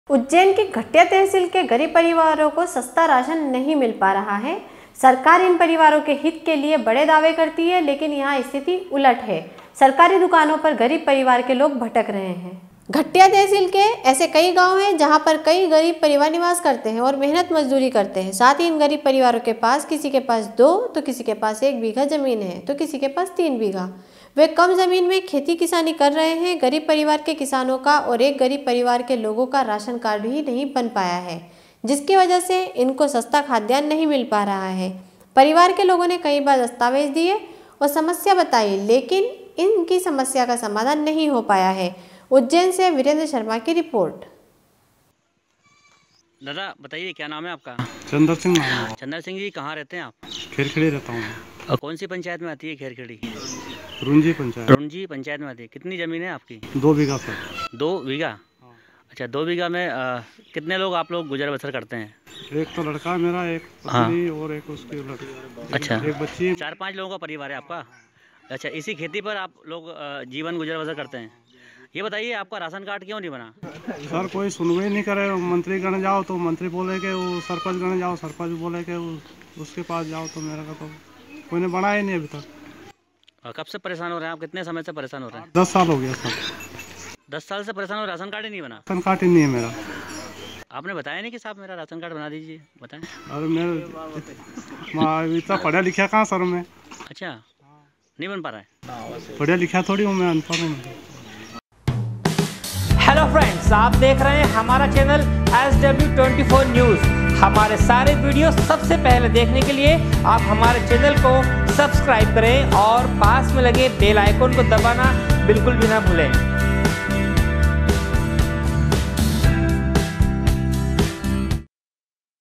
Ujjain- उज्जैन की घट्टिया तहसील के गरीब परिवारों को सस्ता राशन नहीं मिल पा रहा है।
उज्जैन की घट्टिया तहसील के गरीब परिवारों को सस्ता राशन नहीं मिल पा रहा है। सरकार इन परिवारों के हित के लिए बड़े दावे करती है, लेकिन यहां स्थिति उलट है। सरकारी दुकानों पर गरीब परिवार के लोग भटक रहे हैं। उज्जैन से वीरेंद्र शर्मा की रिपोर्ट